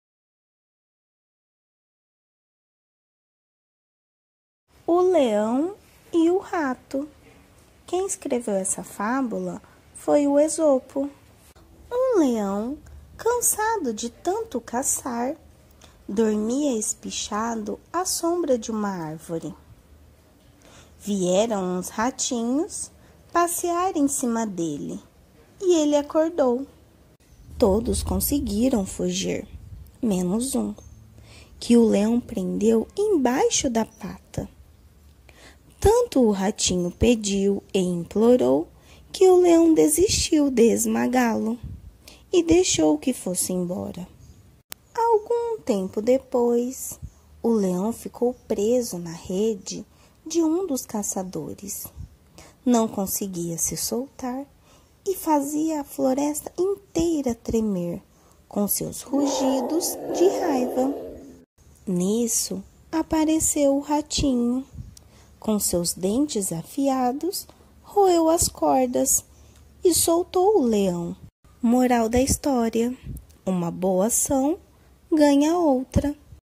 O Leão e o Rato - Esopo
Objetivos: Trabalhar gênero textual: Fábula;
_ Trabalhar a criatividade, os valores, a gentileza e uma boa ação ganha outra.